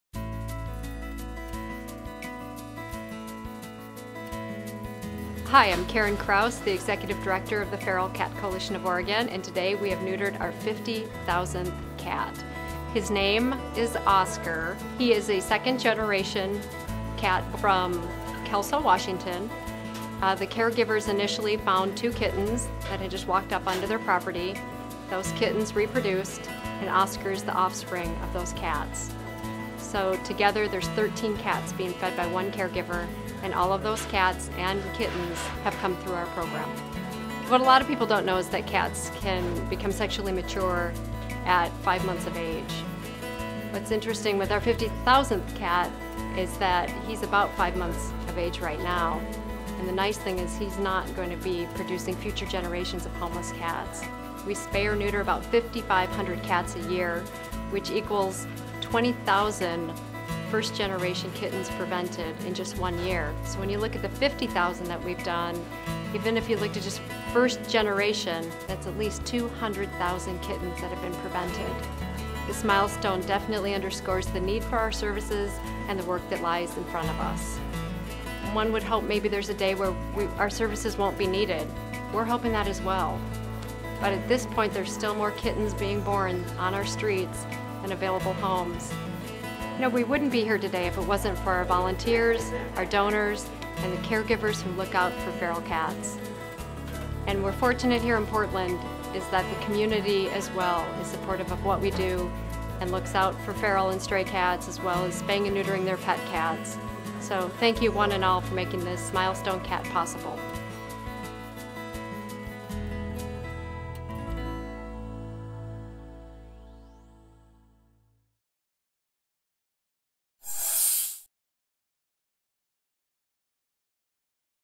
FCC0 50000.mp4
A video featuring the Feral Cat Coalition of Oregon's 50,000th cat helped